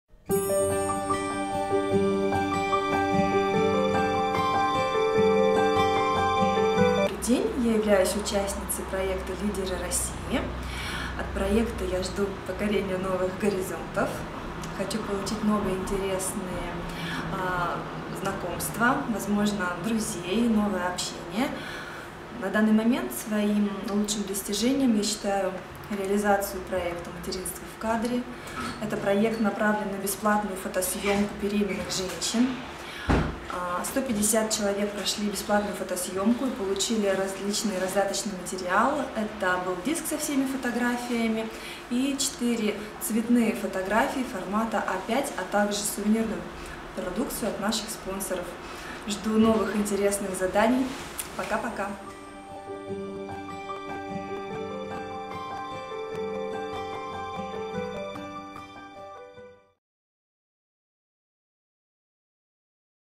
Лидеры россии интервью
Одно из заданий конкурса "Лидеры России" 2019 - это запись интервью в формате видео.